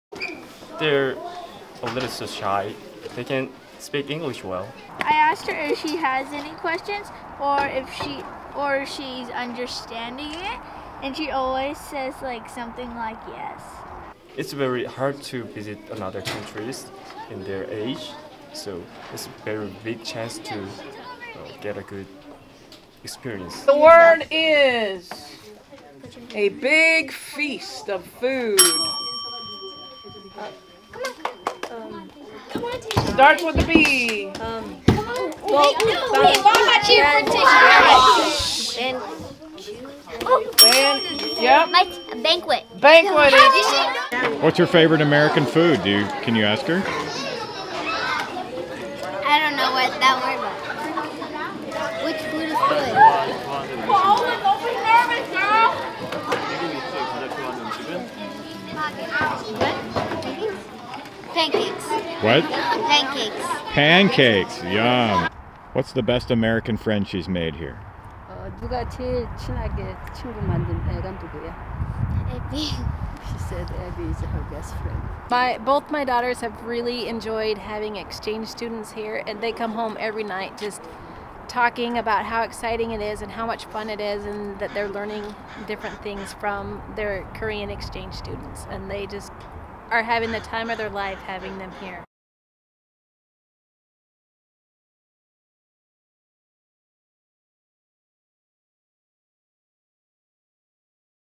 Korean students getting warm welcome at Jennie Reed Elementary
Four students and a teacher from Gunsan, Korea, one of Tacoma's sister cities, have made an extended visit at Jennie Reed Elementary as part of the school's global citizen program. Despite a language barrier, they've befriended American students, played games and experienced the wonder of pancakes.